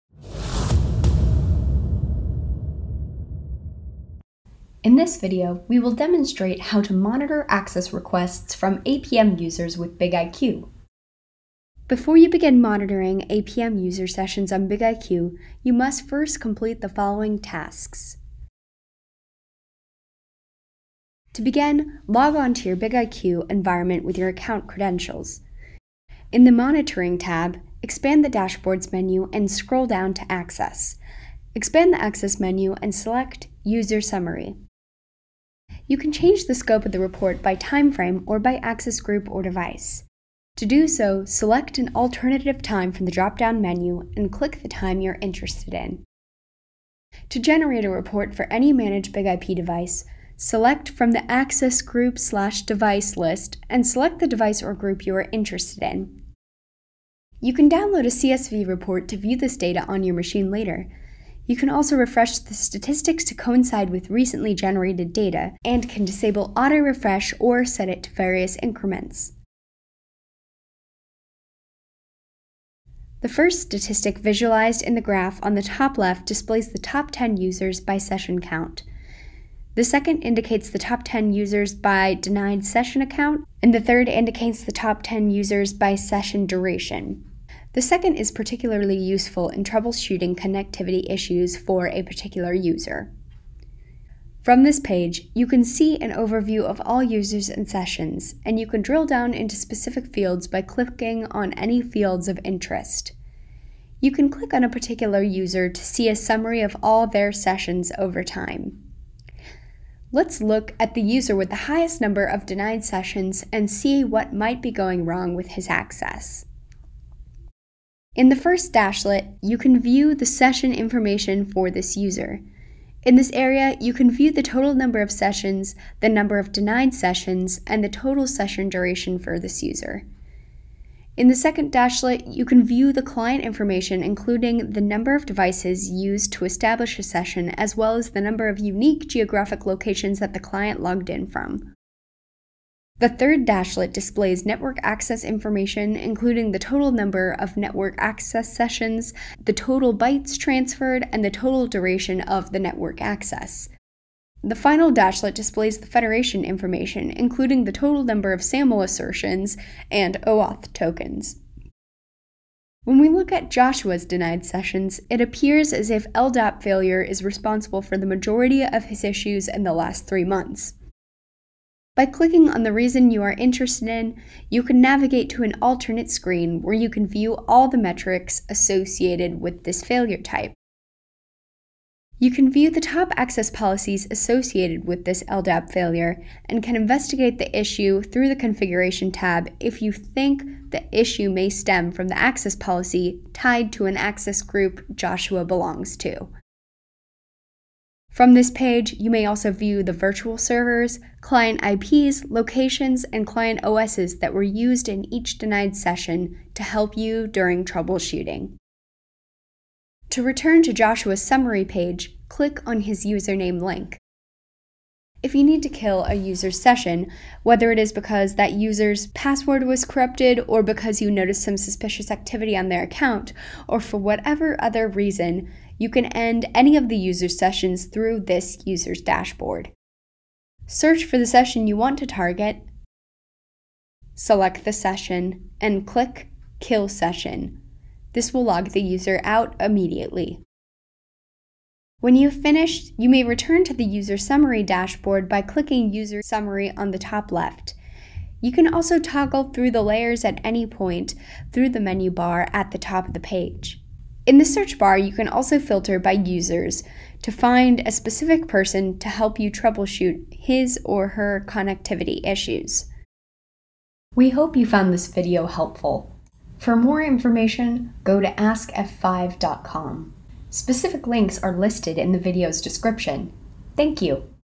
BIG-IQ: How to Monitor BIG-IP APM User Data
This video provides a step-by-step guide on monitoring BIG-IP APM user data using BIG-IQ. It covers the prerequisites for accessing user sessions and demonstrates how to log into your BIG-IQ environment. Viewers will learn essential tasks to effectively monitor access requests from APM users.